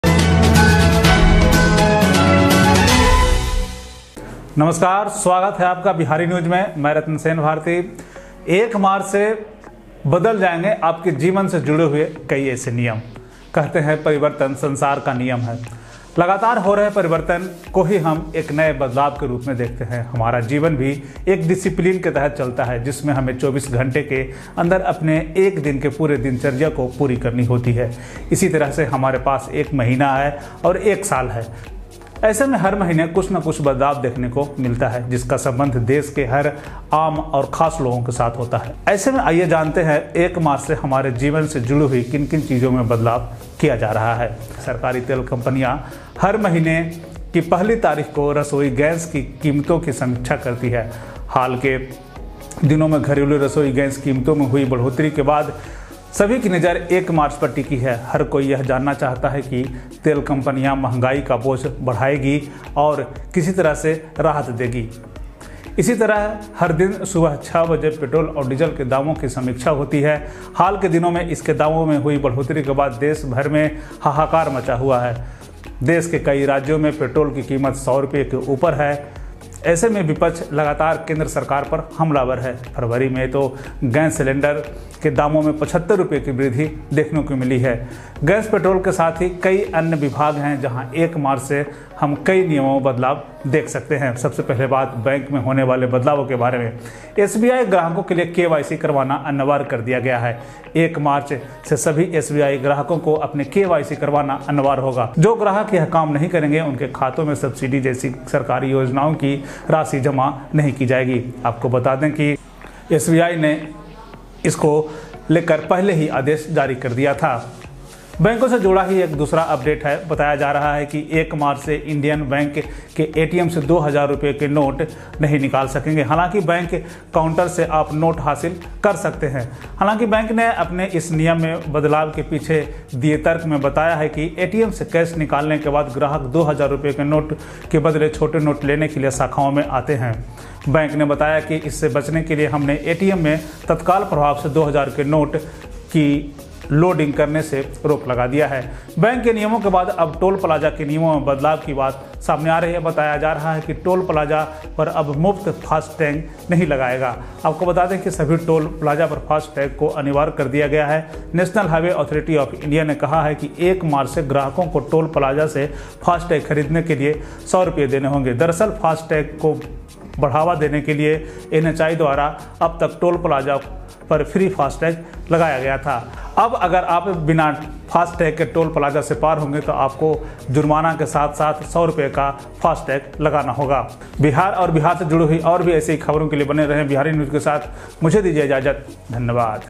1 मार्च से बदल जाएंगे आपके जीवन से जुड़े ये नियम
For the past several days, the prices of petrol, diesel and cooking gas have been steadily increasing. In such a situation, this is going on in the minds of the people that when this price will be reduced. In such a situation, let us tell you that people hope that when the prices of diesel petrol and cooking gas will be reviewed from March 1, the prices will decrease in all of them. Along with this, there is talk of change in SBI in which it is being said that KYC will have to be done. 2000 notes will not be withdrawn from Indian bank ATMs and if you have not installed fasttang at the toll plaza since March 1, then you will be charged fasttag with 100 rupees.